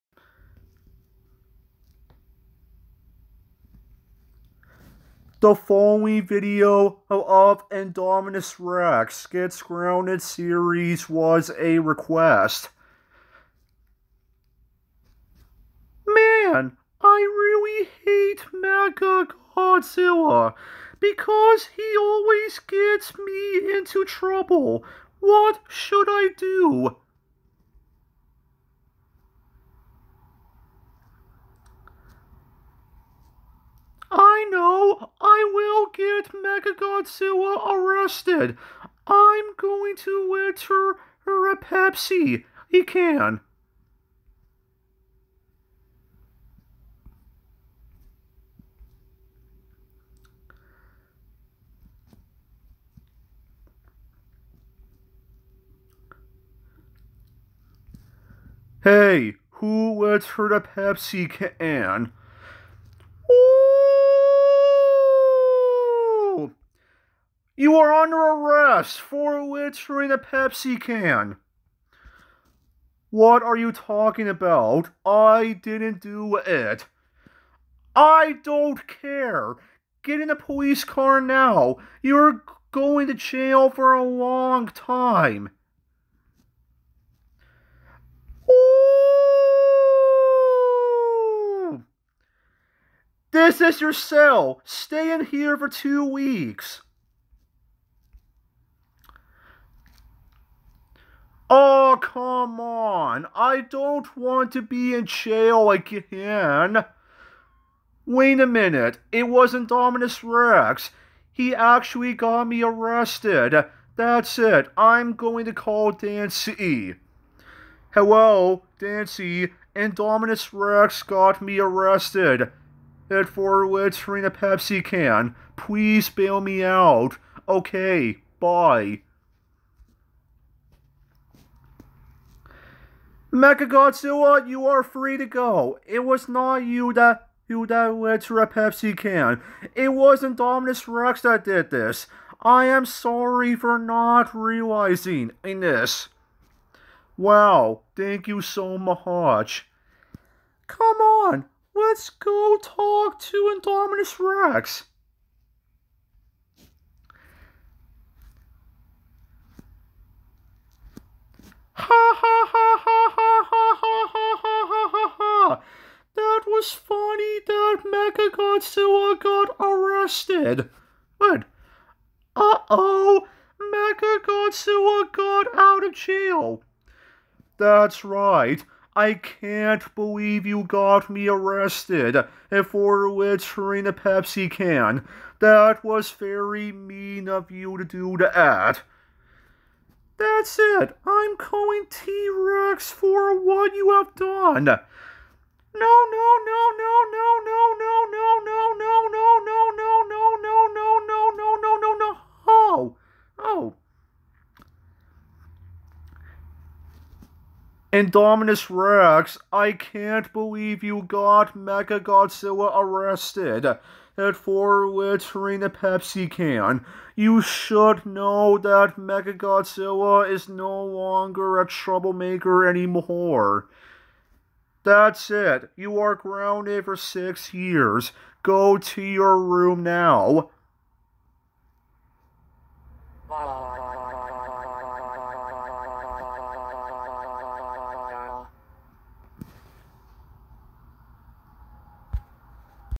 Indominus Rex Gets MechaGodzilla Arrested/Diancie Bails Him out/Grounded
Lesson: Never try to get someone arrested when they didn't do anything wrong in public.